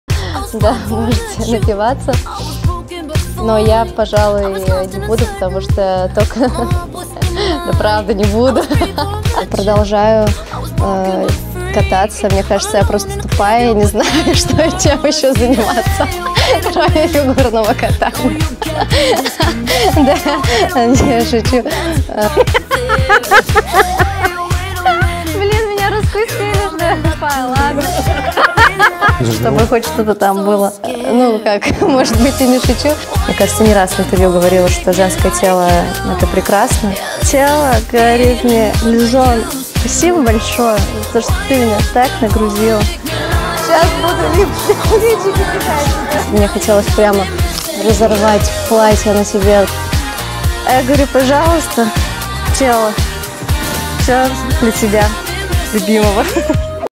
ЕЛИЗАВЕТА ТУКТАМЫШЕВА | TUKTAMYSHEVA'S QUOTES
Елизавета и её удивительный талант вкидывать в интервью запоминающиеся фразочки.